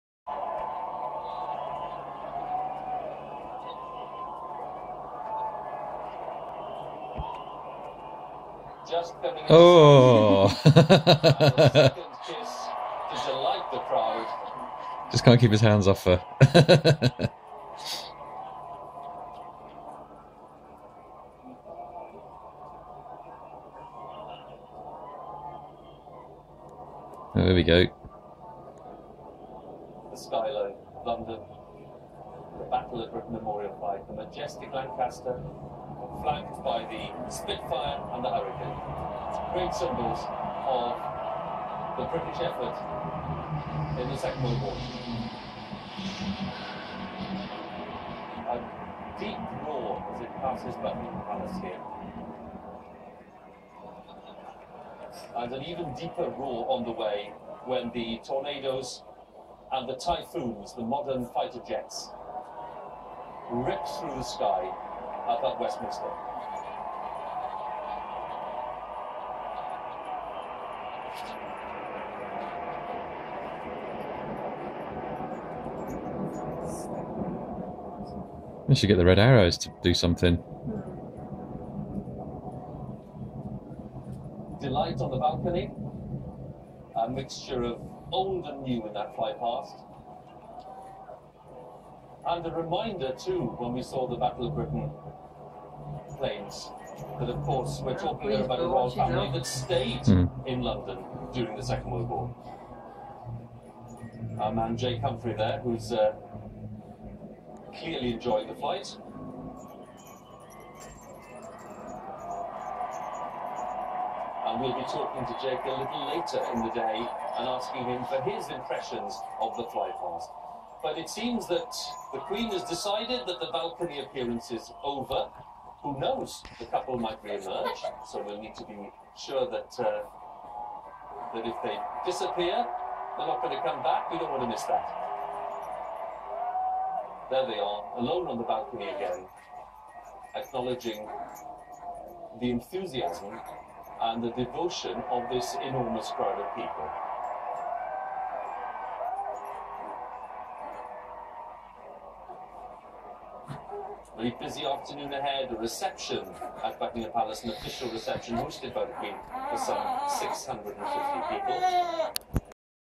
Kate and William balcony kiss + WW2 fly past
The second of two kisses between Kate and William plus Battle of Britain memorial flight and fly past of RAF Tornado and Typhoon fighter jets.